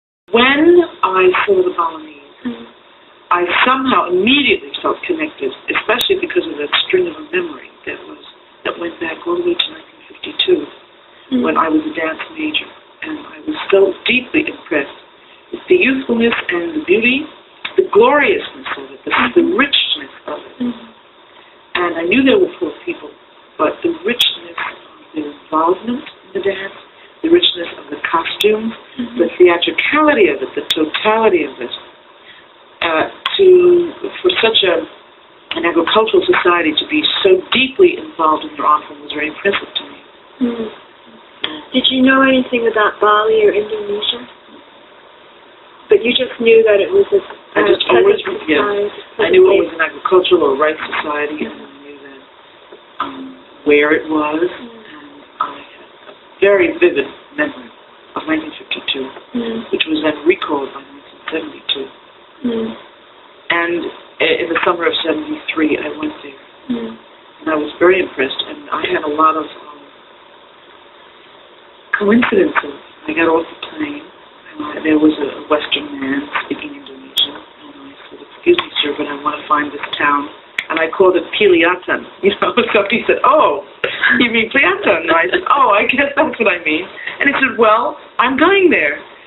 Islene Pinder 1988 Interview excerpt
BALAM's founding director was interviewed many times for her ground breaking and visionary work in Balinese and Balinese inspired fusion dance.  Hear her passion and intelligence in this 90 second excerpt as she tells the berginning of her story.